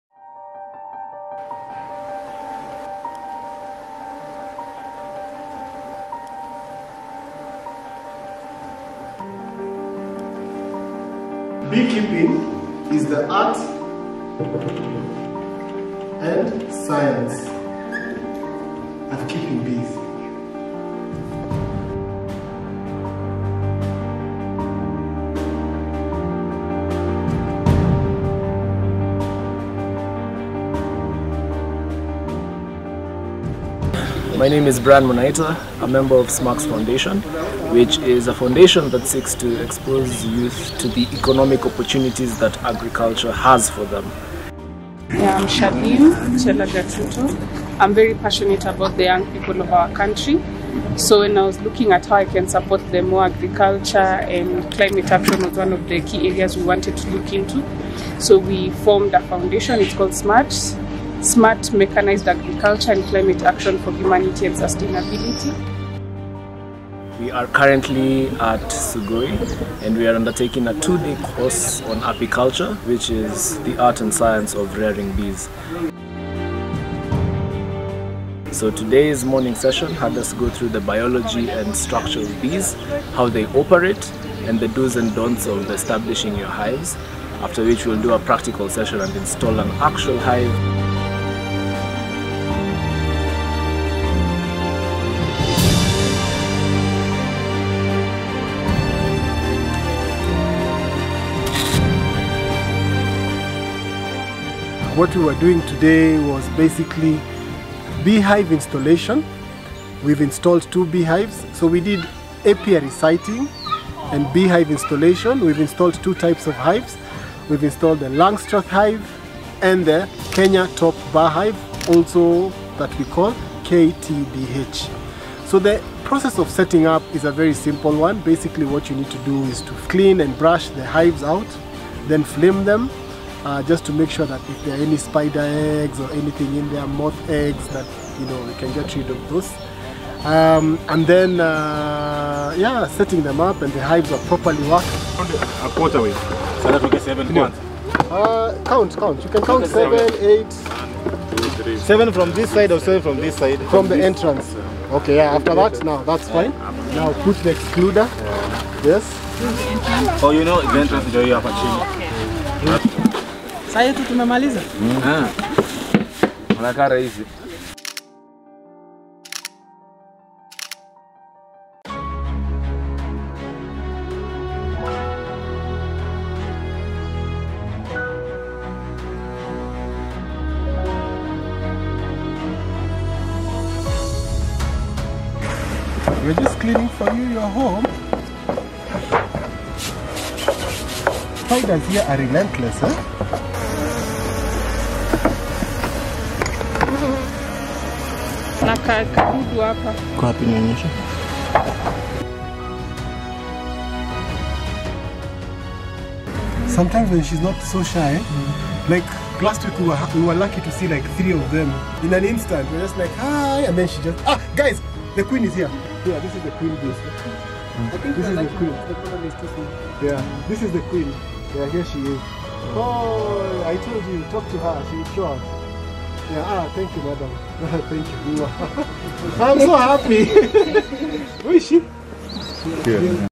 Beekeeping 101: A Short Visual Guide | SMACHS Foundation
In honor of WORLD BEE DAY, join us as we advocate for beekeeping and its crucial role in sustainable agriculture in this captivating video. As a foundation committed to promoting the welfare of bees, we delve into the fascinating world of beekeeping, sharing insights and knowledge that empower individuals to become beekeepers themselves. Discover how setting up hives, tending to bees, and extracting honey contribute to the preservation of these vital pollinators and the health of our ecosystems. Through this video, we aim to inspire and educate, fostering a greater understanding of the importance of beekeeping in building a sustainable future. Embrace the journey with us and join the movement to protect and support bees for generations to come.